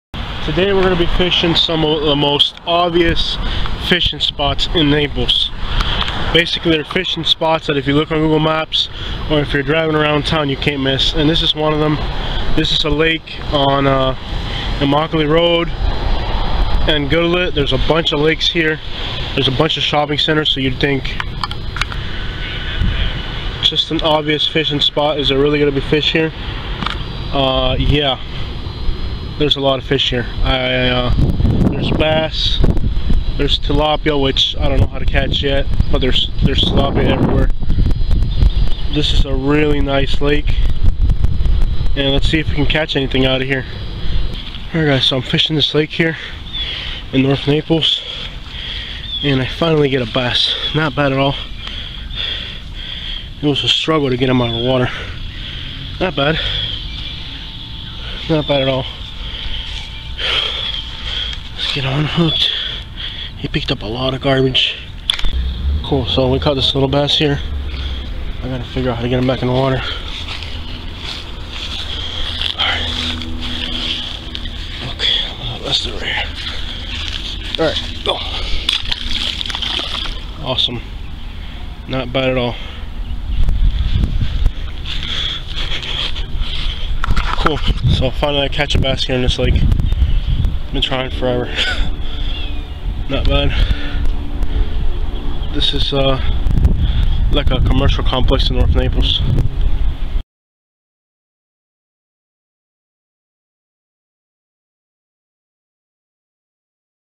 Most Obvious Fishing Spots In Naples, Florida
never overlook a pond because it is right in the middle of a shopping center!